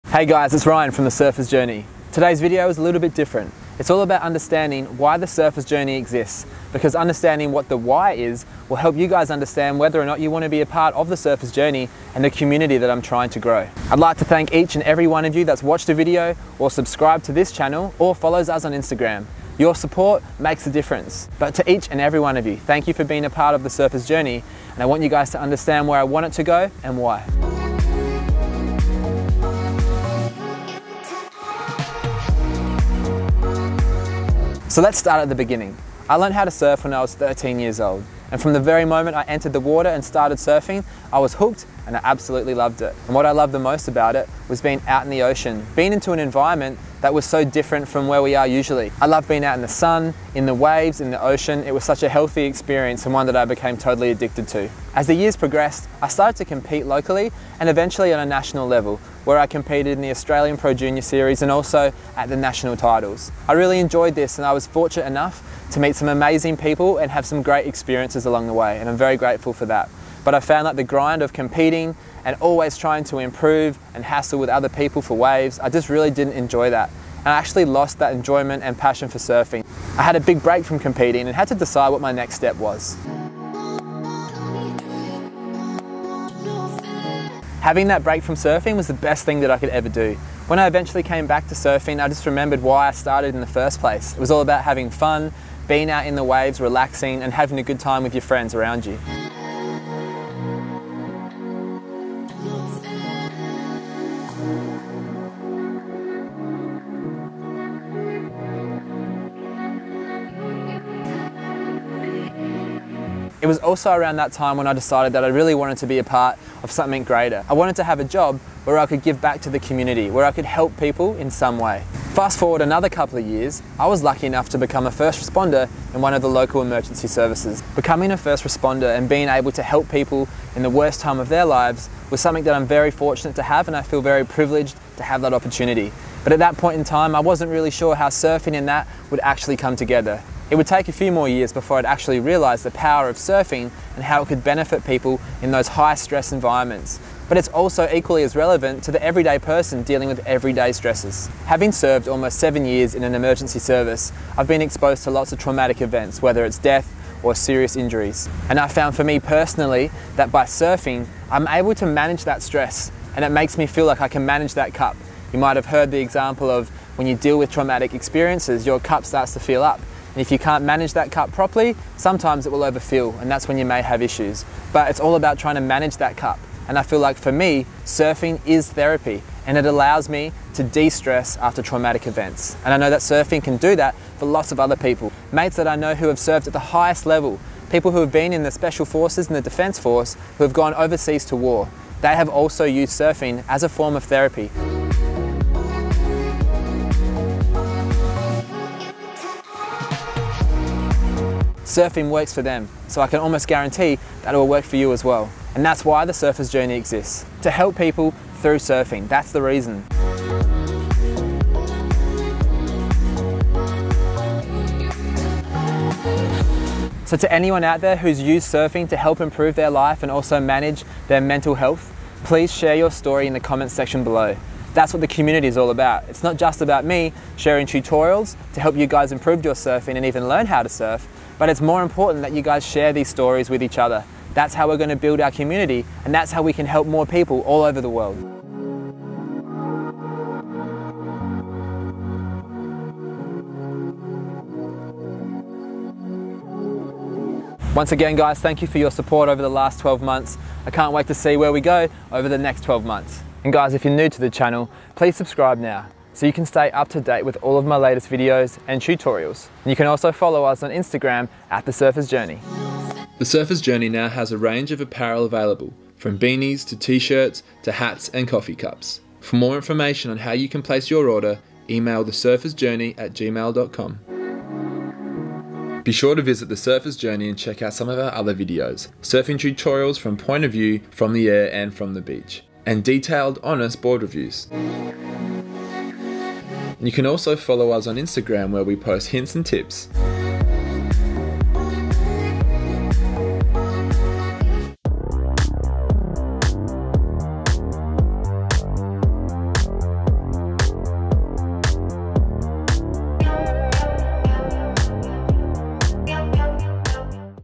Surf and Serve | One Surfers Dream To Save Lives
Why does The Surfers Journey exist and what is its purpose? To mark our first birthday we explain exactly who we are and where we want to go.